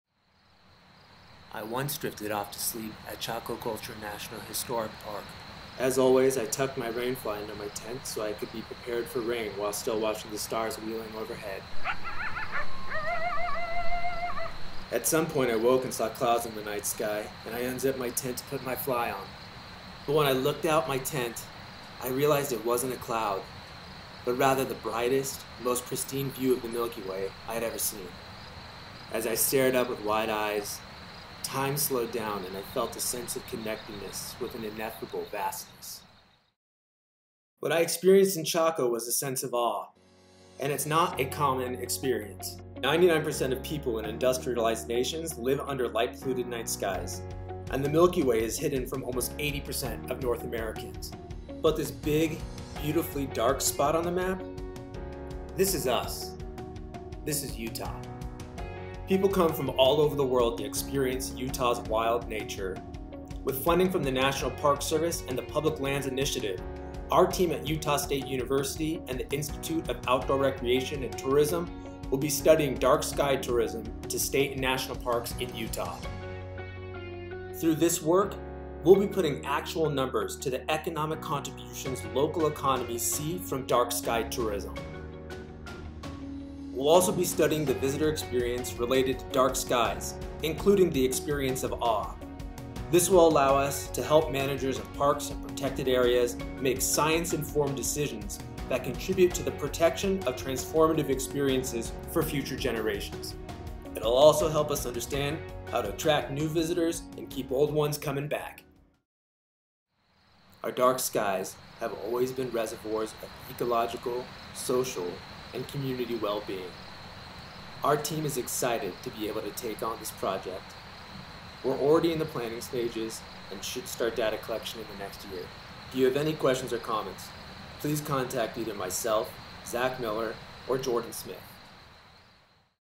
IORT Research: The Utah Dark Skies Research Project
Learn more about an upcoming research project focused on the economic benefits of dark skies. Funded through the National Park Service and the Public Lands Initiative